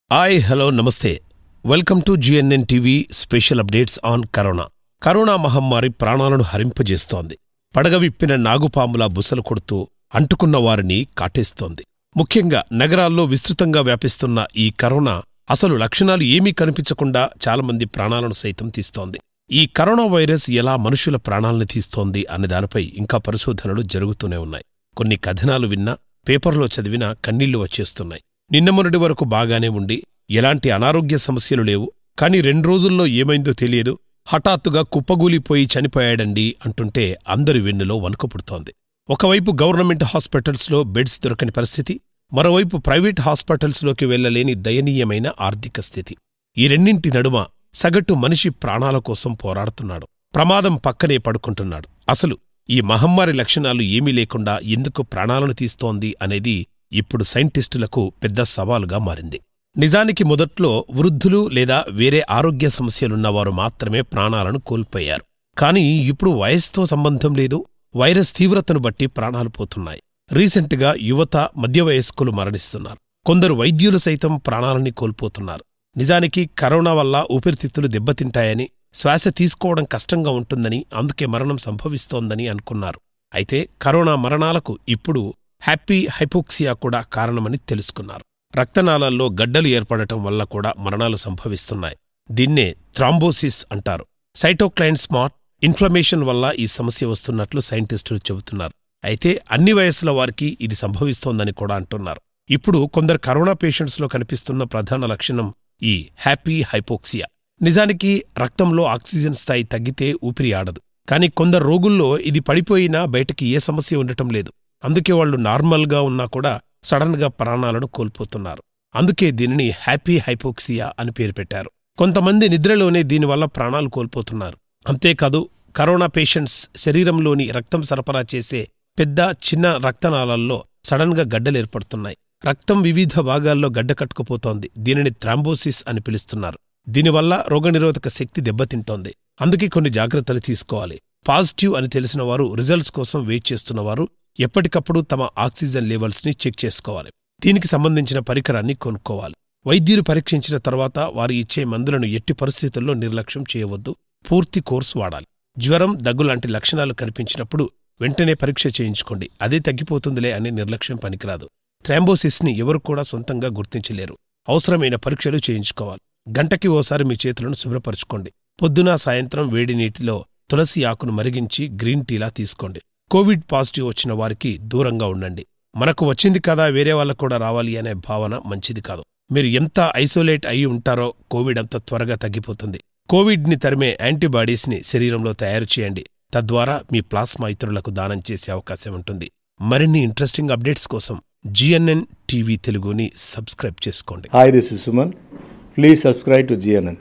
Asymptomatic Covid-19 Symptoms Explained 🙏😱| కరోనా లక్షణాలు లేకుండానే ప్రాణాలు పోతున్నాయా? | GNN TV
Shocking Symptoms Of Corona | Asymptomatic Covid-19 Symptoms Explained | కరోనా లక్షణాలు లేకుండానే ప్రాణాలు పోతున్నాయా? | Corona New Symptoms